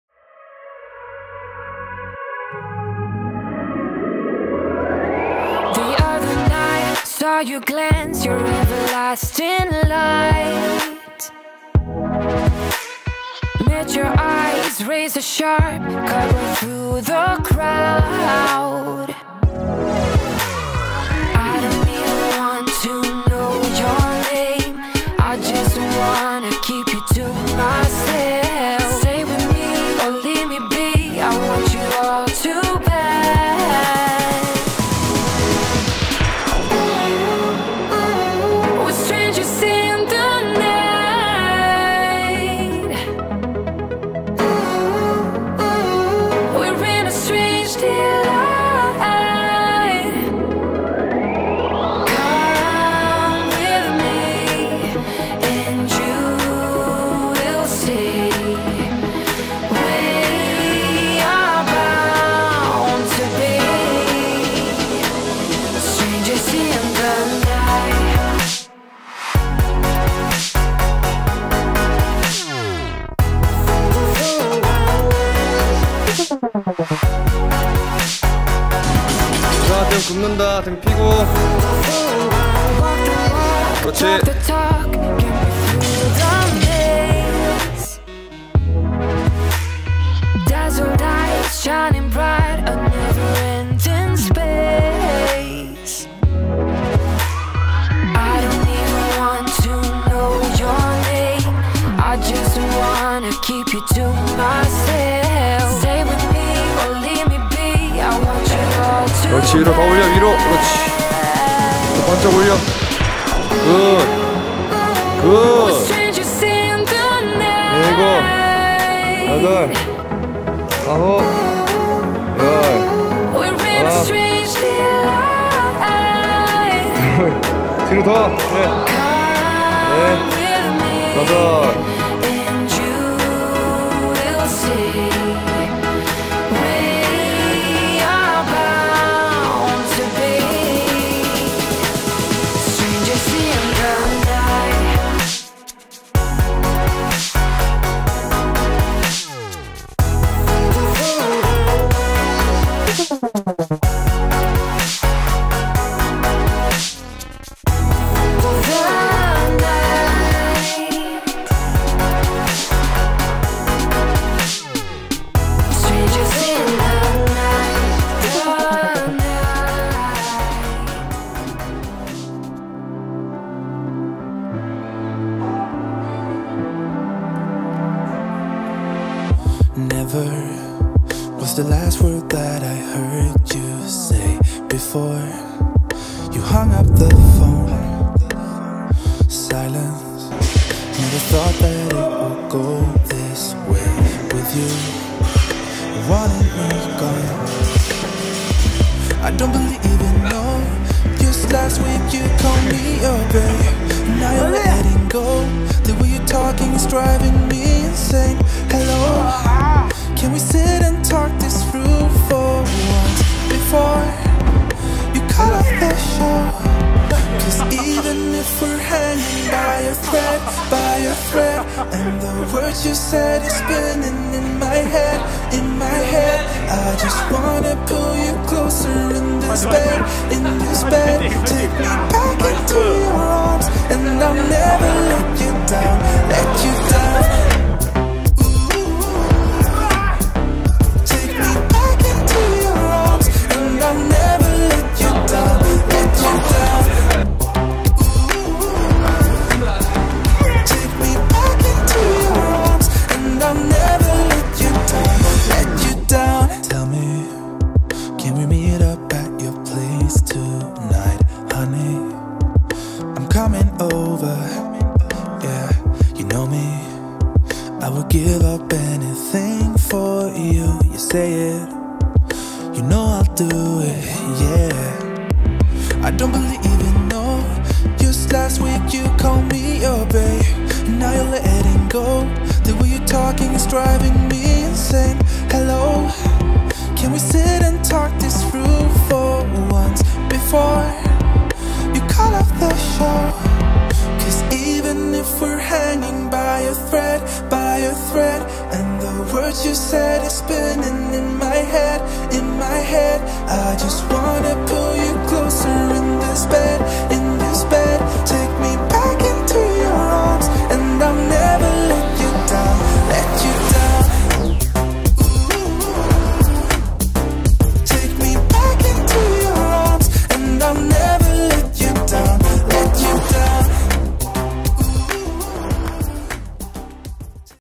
해성이와 야무지게 5x5 (운동 자극)
간만에 찾아온 해성이와 운동했어요~